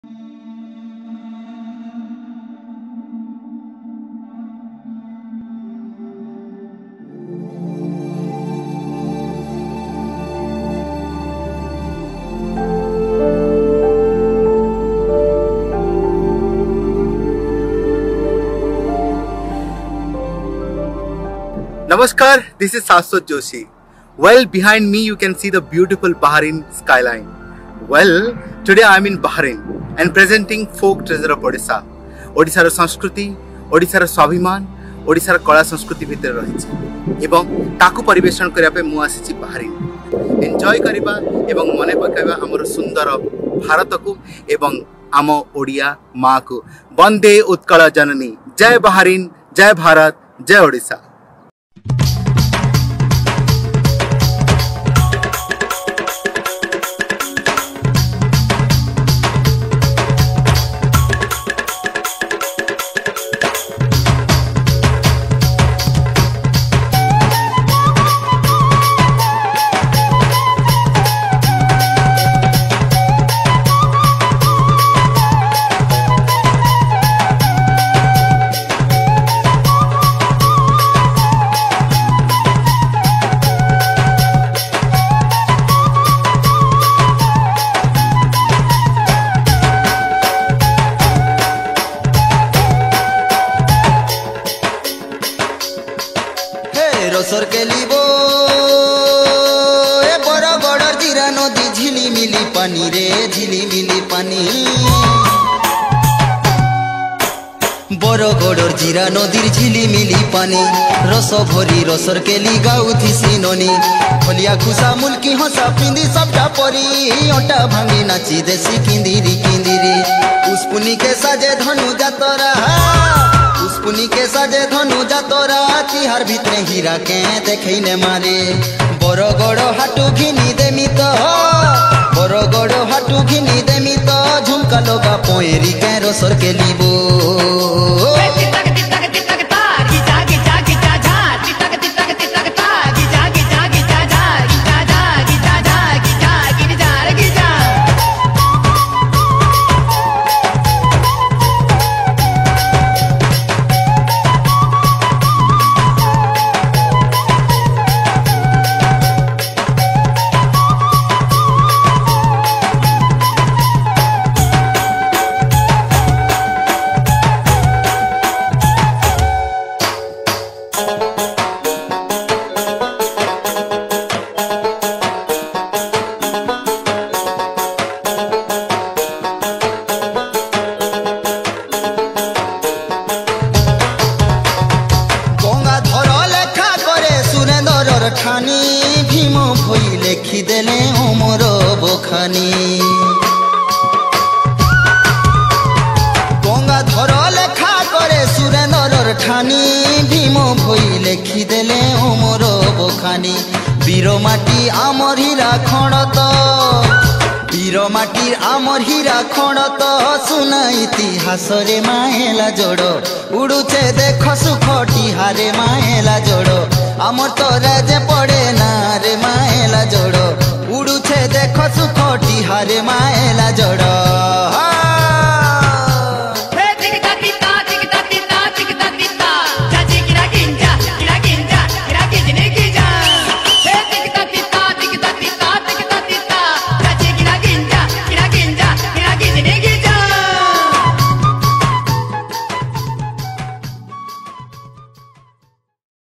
Sambalpuri Dance In front Of Bahrain Skylines || India In Bahrain || 50years united In friendship
It’s absolutely a tribute to the beautiful Land India from Bahrain, I was invited as guest artist for the Utkal Divas celebration 28th April with Sagarika Mohanty and Sonal Acharya ,organised by Bahrain Odia Samaj headed by Shri Arun Kumar Praharaj, so as India and Bahrain celebrating 50years of Friendship , we (Team Saswat Joshi) gave a tribute. Respect all the artist who created this music , respect all the singers and musicians who composed this art form in Sambalpuri, Nachnian and Maelajada…

It’s my sincere tribute to G20 @G20ORG @MinistryofCultureGovtofIndia and my India